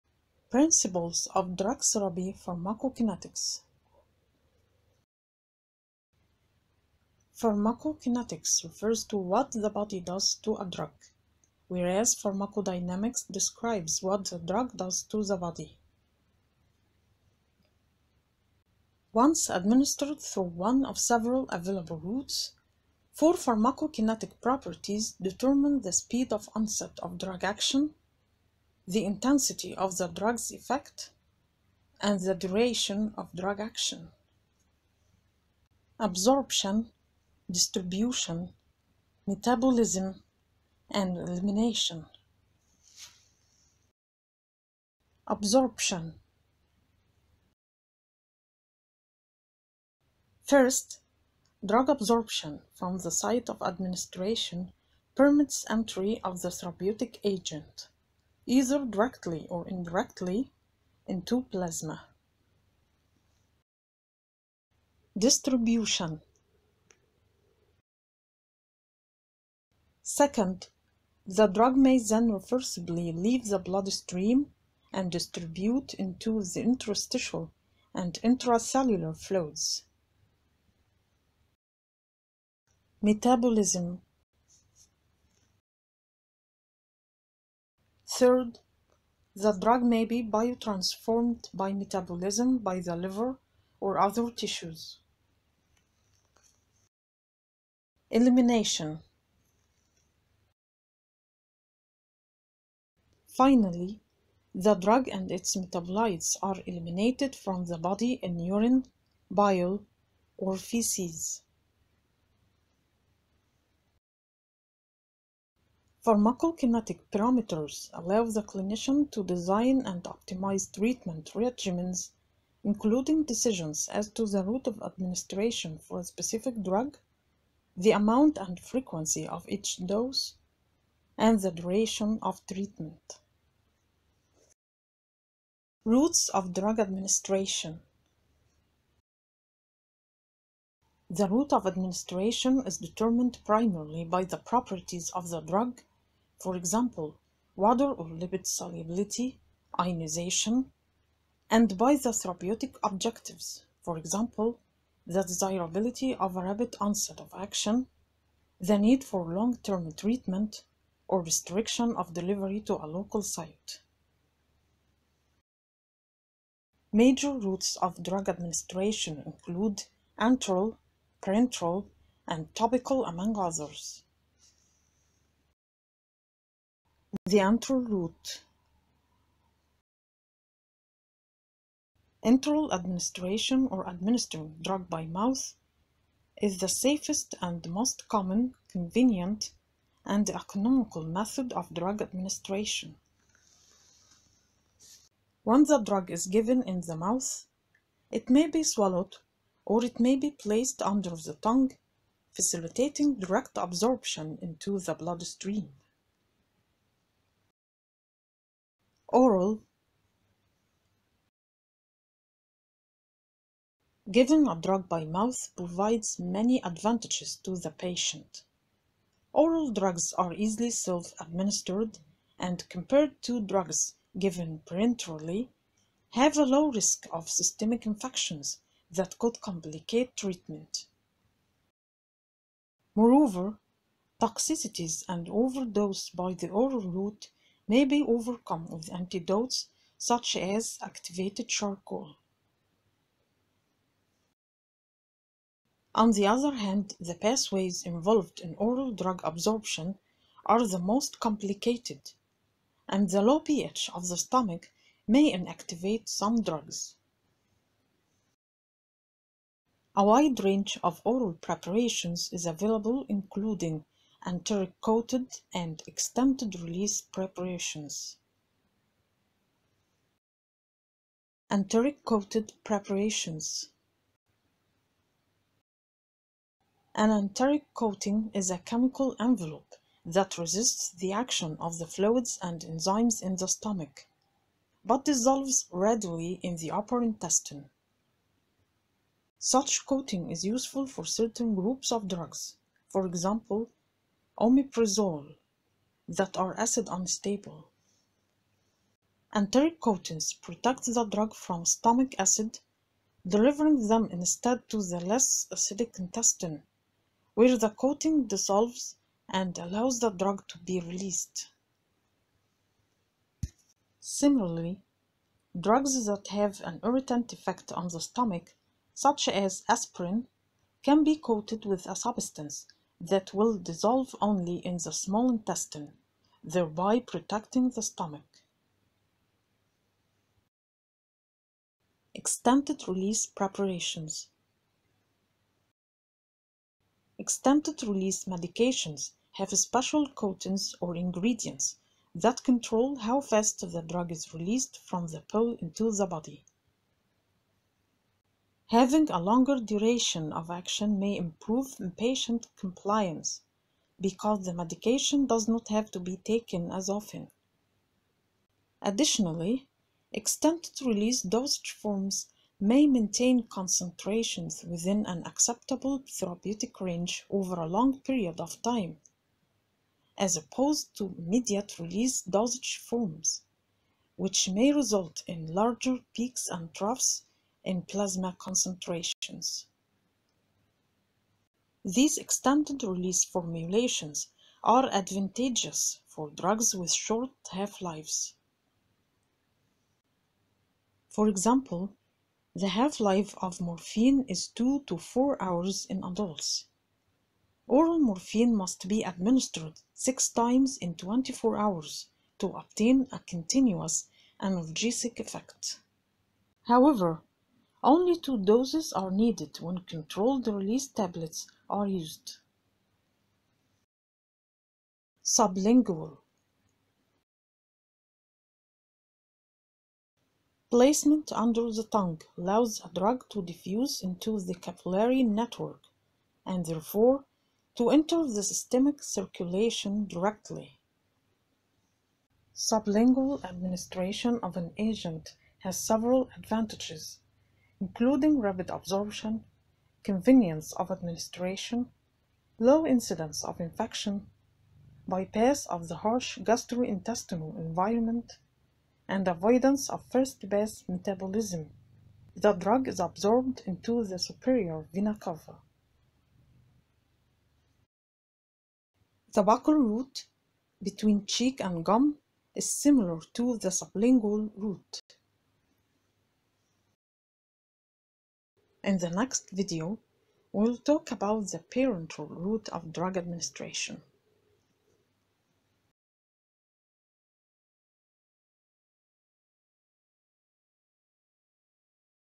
Pharmacokinetics - The Enteral Route of Drug Administration
This video is translated into Arabic and it discusses:

♦️Principles of Drug Therapy
Pharmacokinetics

♦️Pharmacokinetics vs pharmacodynamics
♦️Absorption, distribution, metabolism, and elimination
🔵The Enteral Route of Drug Administration
oral, sublingual, and buccal routes of administration

Reference: Lippinecott's Illustrated Reviews - Pharmacology